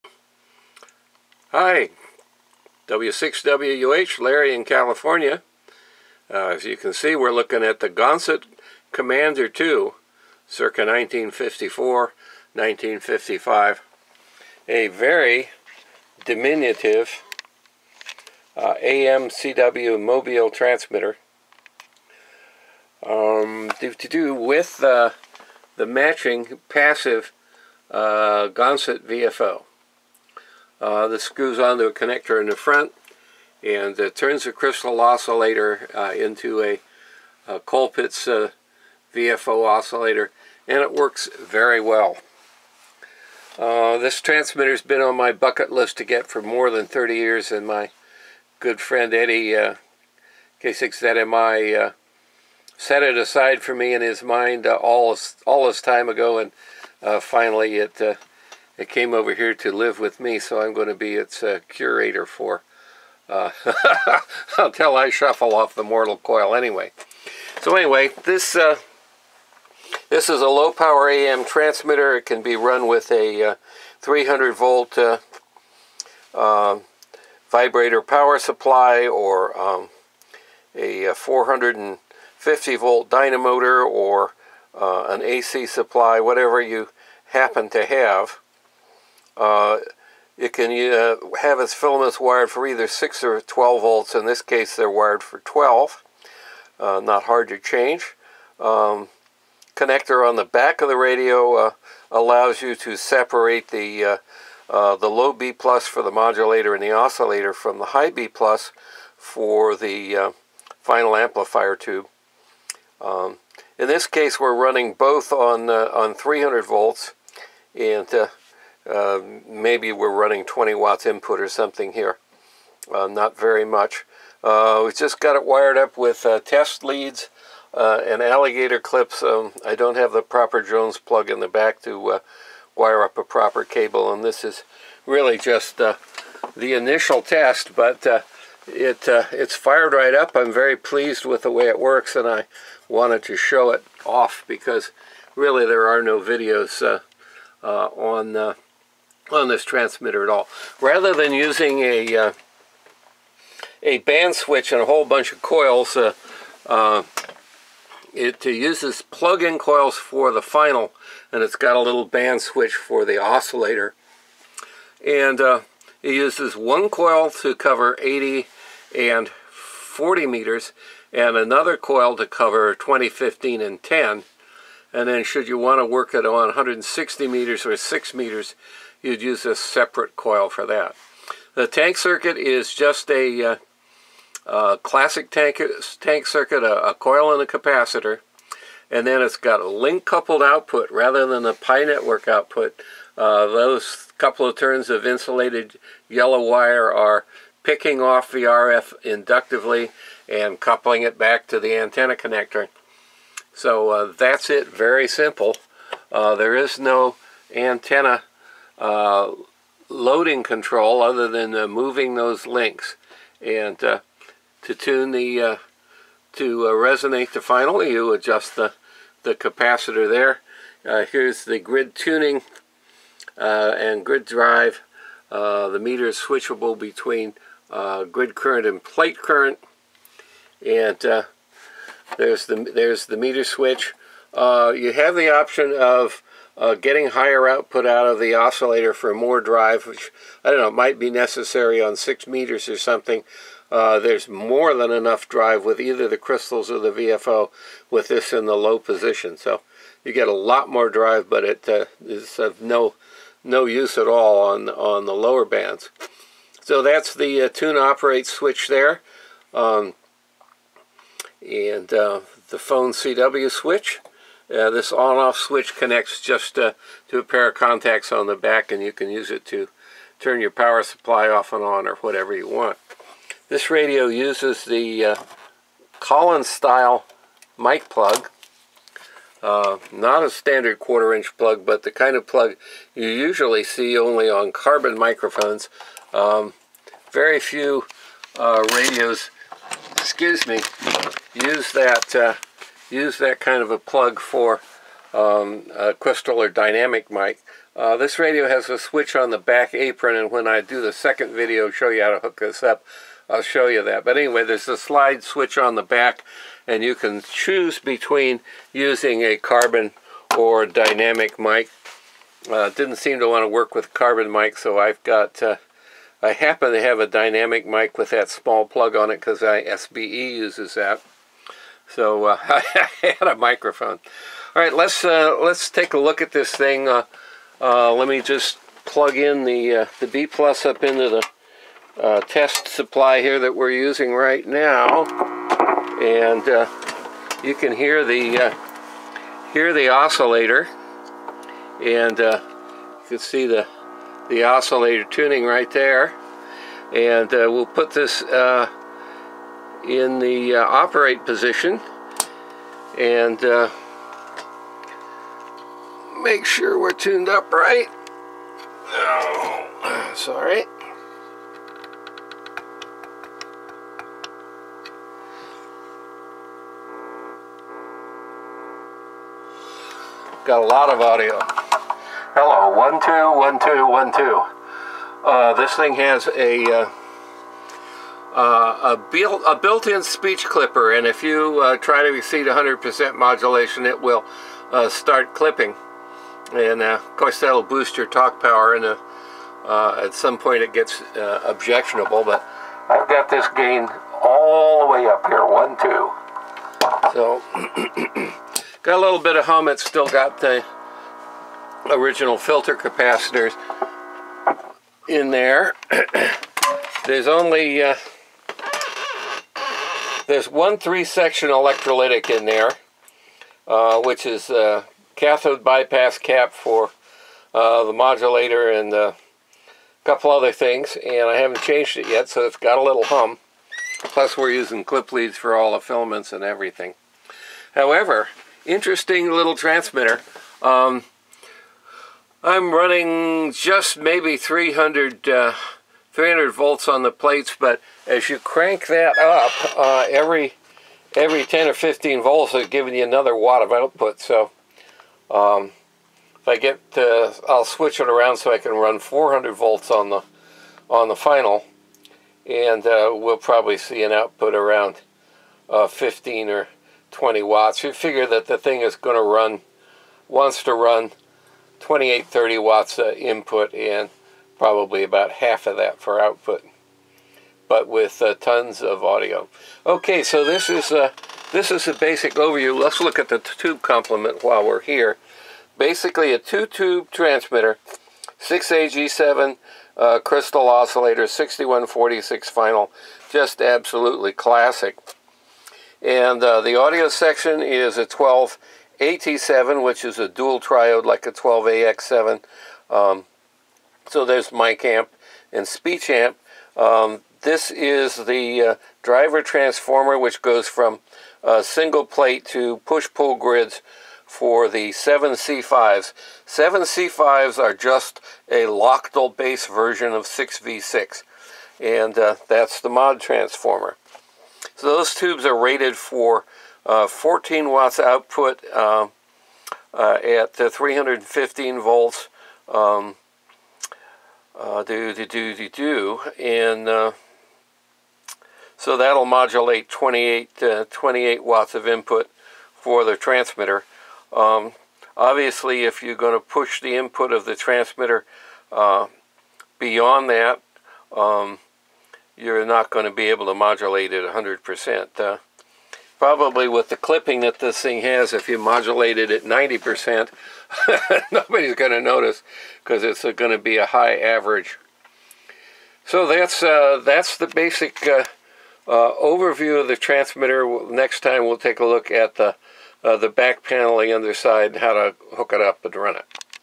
W6WUH Inside Classic Ham Radios: Gonset Commander II Mobile AM Transmitter
Narration in this 1st video. may contain mistakes !.. please review videos 2-5.
Overview of the circa 1954 Gonset Commander II AM/CW Mobile Transmitter and matching passive VFO.  This rather rare and diminutive mobile transmitter has an excellent Push Pull Modulator and a 6146 final, chosen because it can run at very good efficiency over a wide range of plate voltages 300 to 425 Vdc.  The transmitter uses rising impedence mis-match to induce peak clipping and limit modulation to 100 %.  Audio is not clipped unless audio gain is intentionally set to a higher than " normal" setting.  At this power level ( 7-20 watts output)  the higher average modulation gained by some degree of clipping is a welcome advantage.